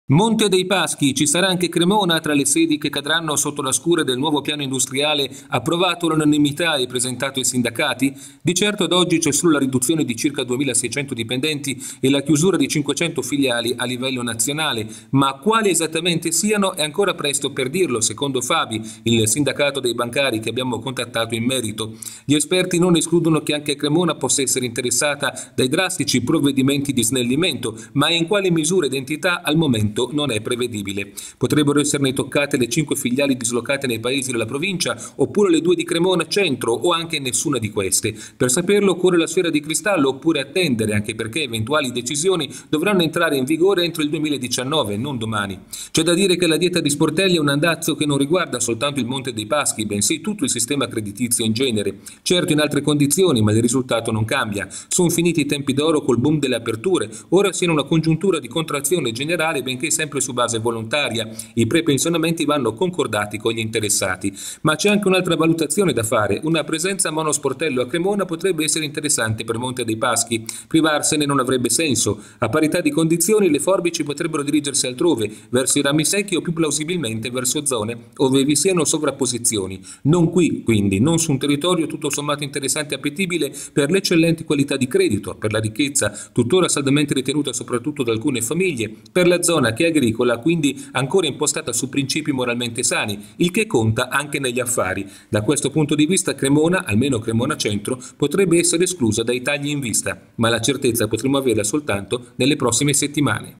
Mps: anche Cremona tra le sedi a rischio tagli?
Monte dei Paschi di Siena: il piano industriale, recentemente approvato, parla di una riduzione di circa 2.600 dipendenti e della chiusura di 500 filiali in tutta Italia. Ci sarà anche Cremona tra queste? E' presto per dirlo, ma di certo ci sono ragioni per restare...